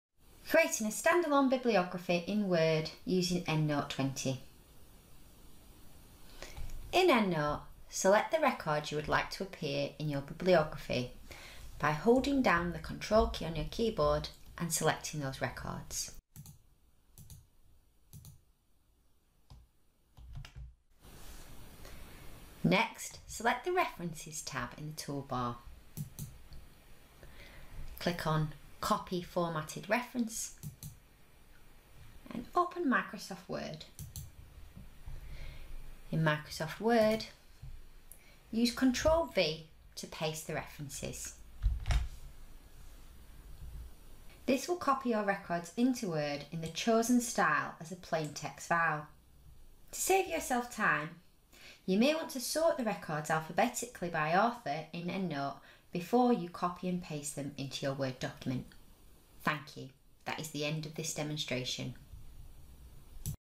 Creating a stand alone bibliography in Word
Creating a stand alone bibliography in Word. This demonstration uses EndNote 20 on a Windows operating system.